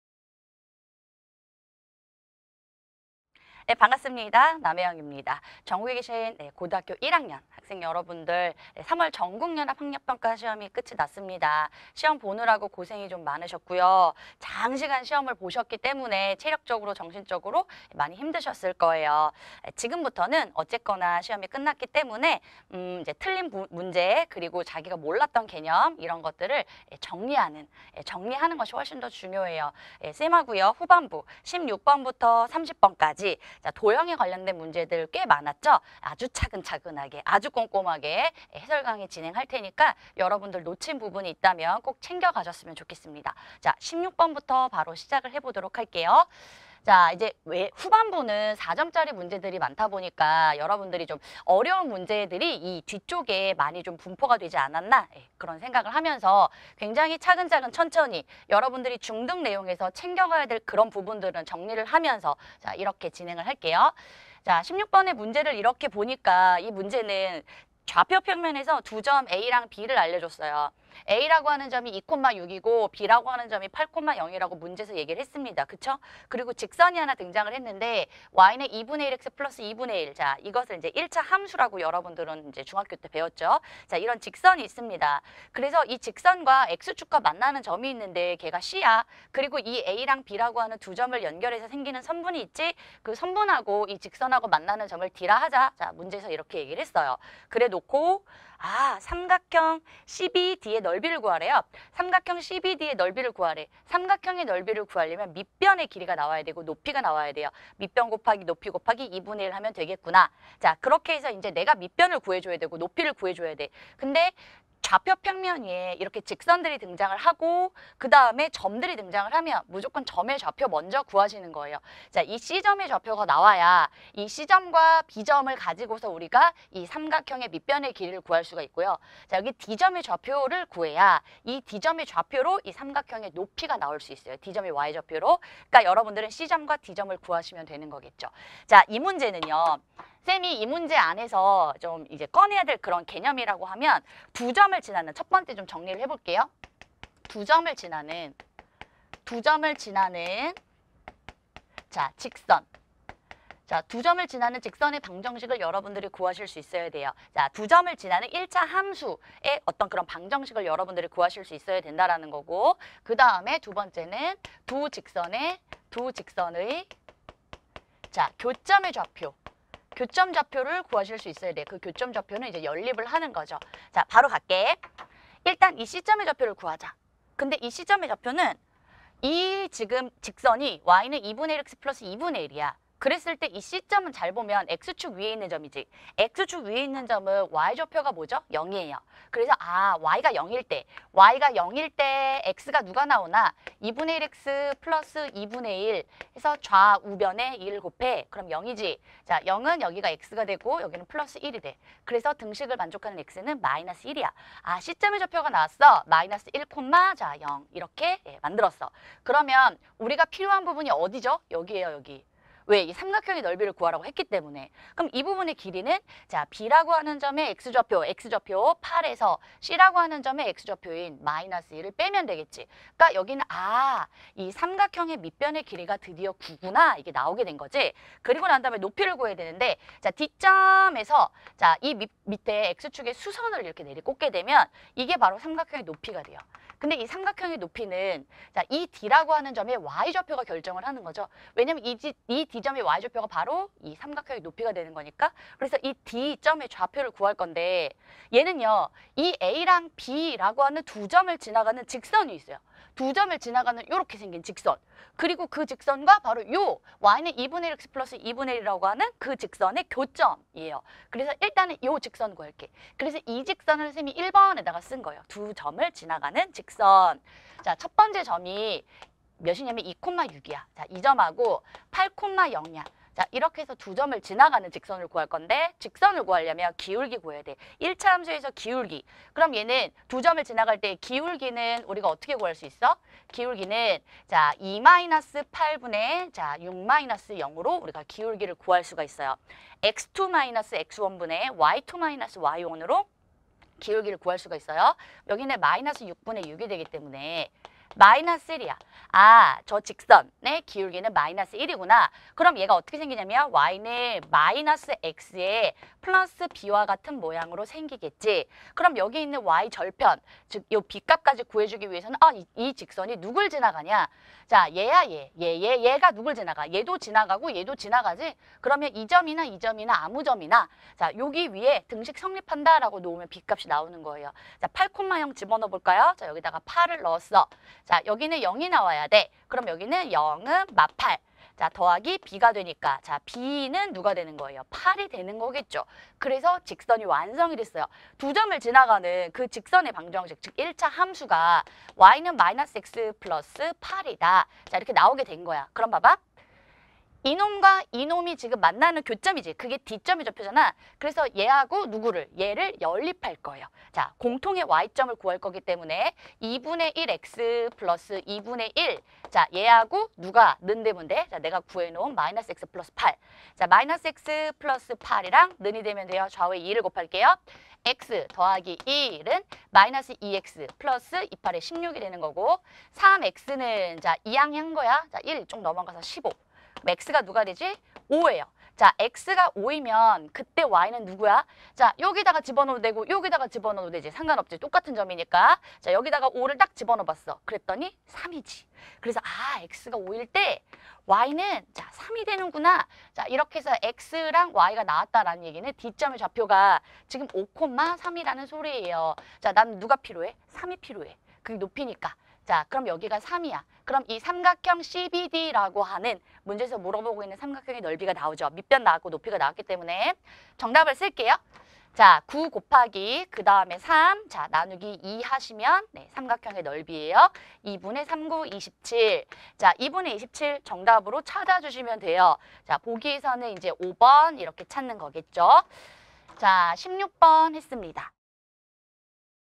[메가스터디] 수학 남혜영쌤-2019 3월 학평 ★수학 해설강의★
2019 3월 학평 수학 해설강의
지금 남혜영 선생님과 확인하세요

-  남혜영 선생님의 강의가 더 궁금하다면?